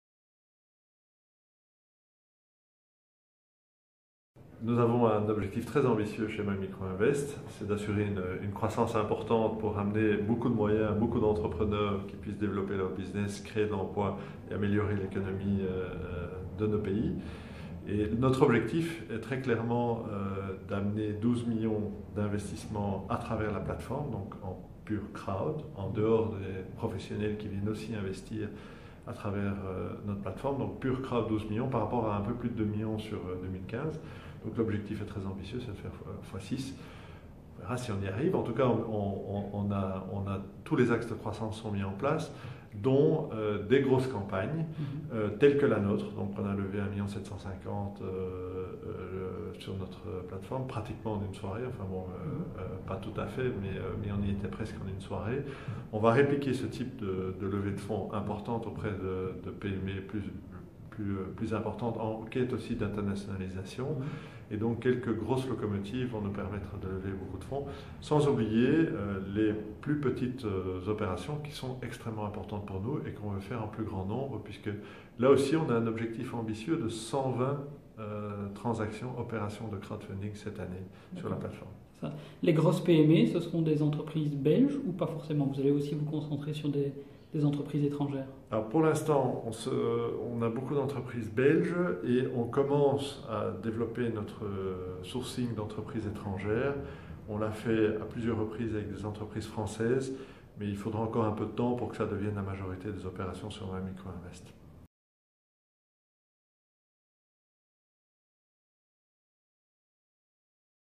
Les objectifs 2016 de MyMicroInvest
Olivier de Duve (CEO) détaille les ambitions du spécialiste du crowdfunding en Belgique.